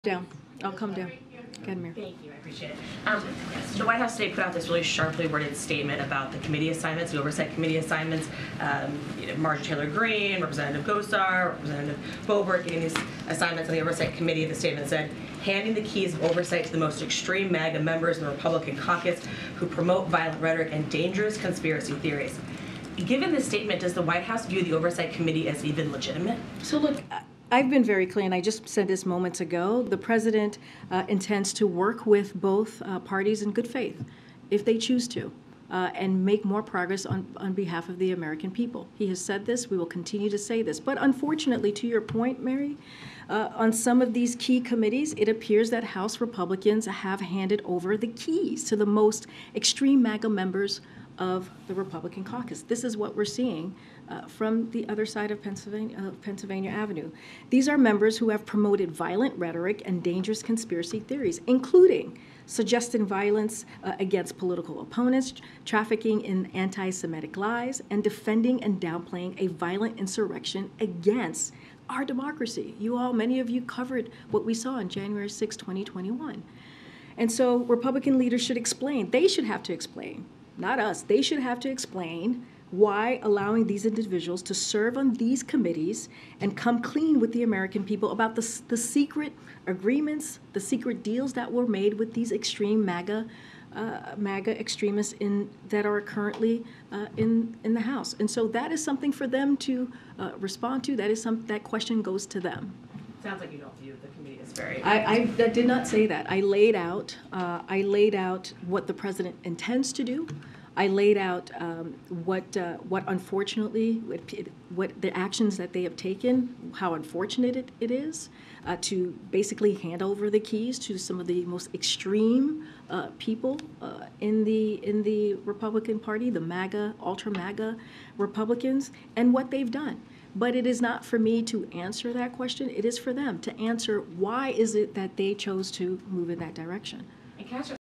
White House Responds To Marjorie Taylor Greene Getting House Oversight Committee Assignment
Karine Jean-Pierre responds to Rep. Marjorie Taylor Greene (R-GA) getting seated on the House Oversight Committee.